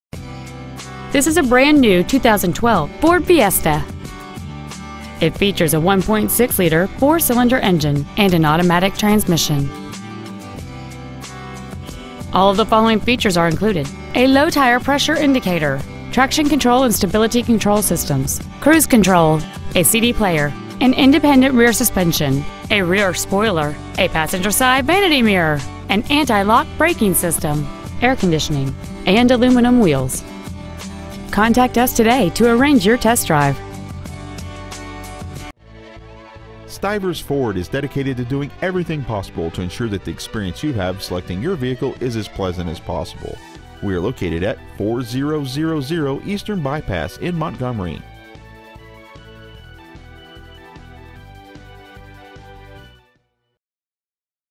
2012 FORD FIESTA Montgomery AL
Year : 2012
 Make : FORD
 Model : FIESTA
 Engine : 1.6L I4 16V MPFI DOHC
 Trans . : AUTOMATIC
 Exterior : WHITE
 Miles : 50
 Interior : CHARCOAL BLACK / LIGHT STONE
 Stock : CY95000